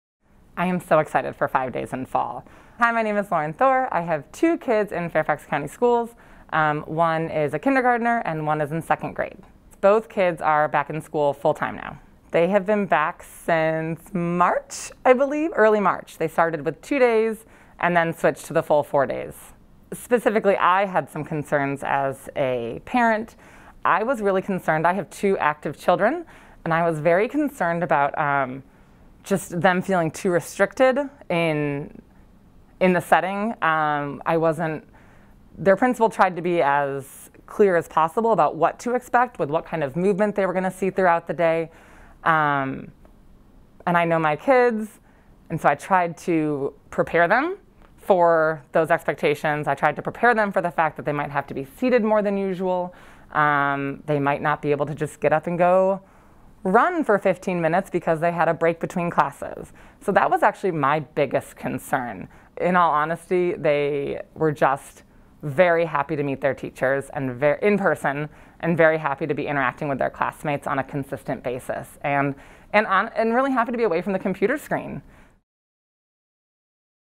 Return to School Parent Perspectives - Lauren Thor
Lauren's two young children are back to in-person instruction. While she had some concerns about the new learning environment, her energetic kiddos have had a great experience returning to the classroom!